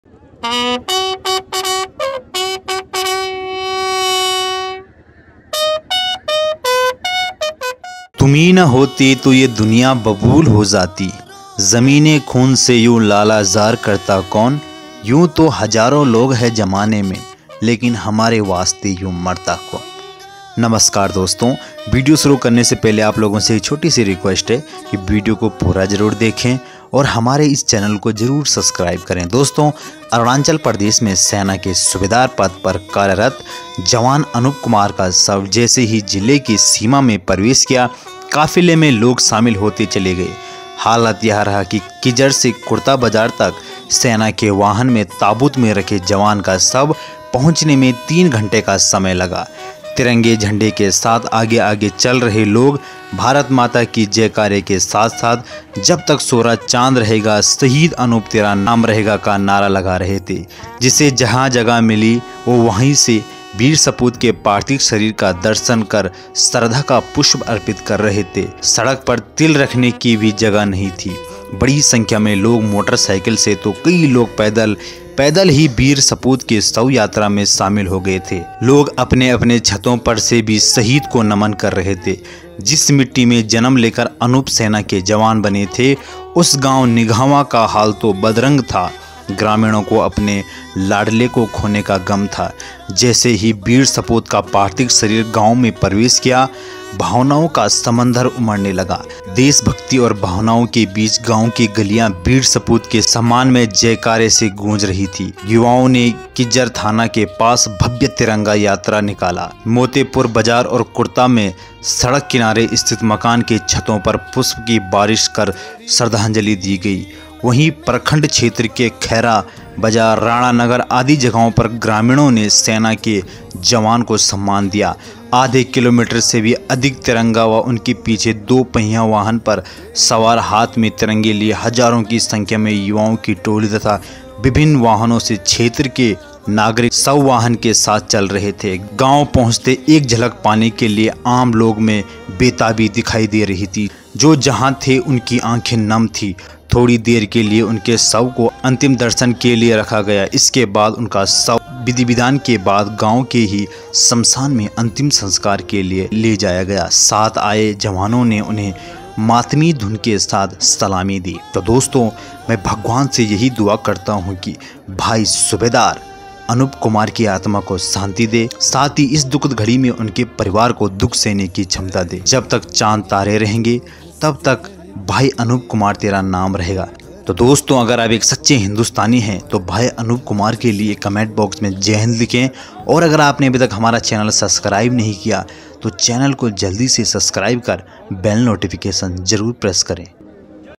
शहीद सूबेदार अनूप कुमार कि अंतिम विदाई || हजारों लोगों ने दी शहीद को अंतिम विदाई || Indian army
🎯like_share_and_subscribe जरूर करें 🙏🥰